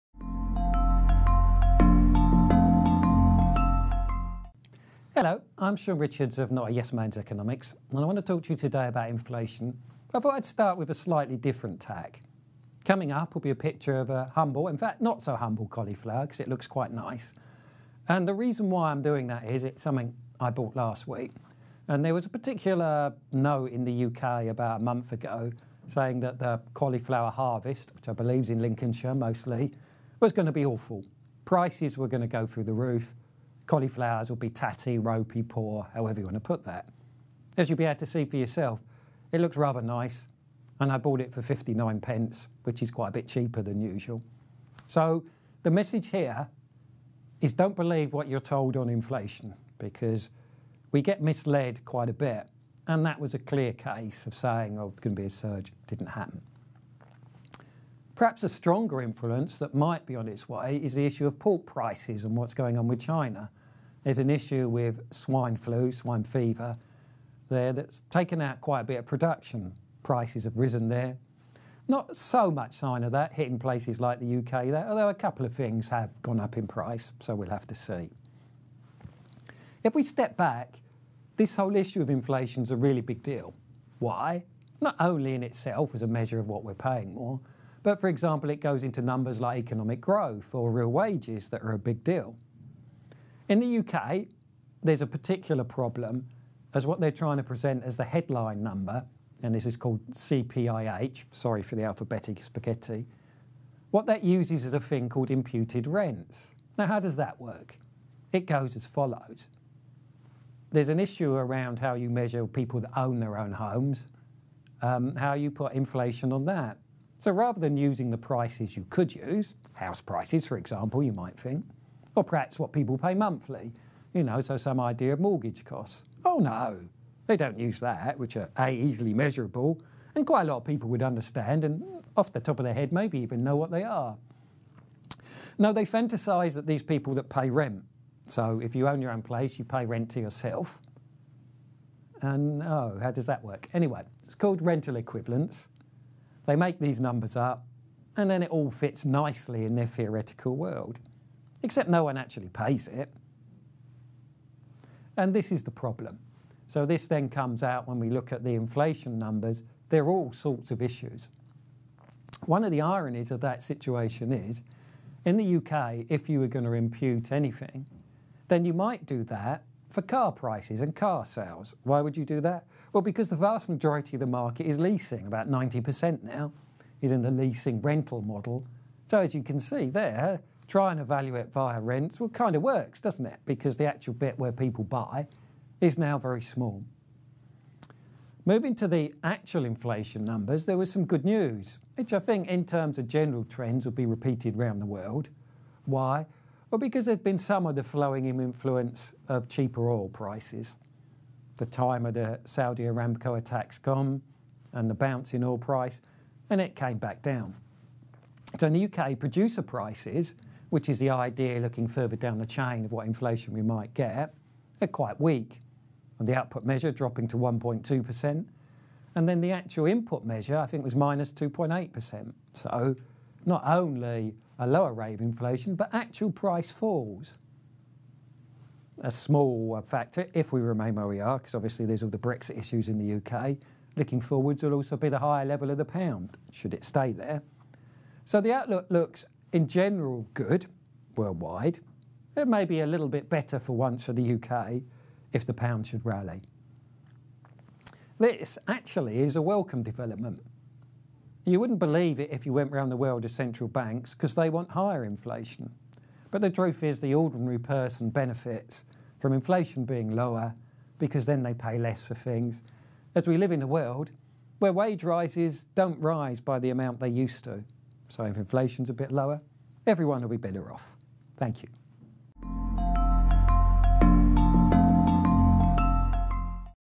Don’t Believe What You’re Told On Inflation
Shaun Richards of Notayesmanseconomics looks closely at inflation.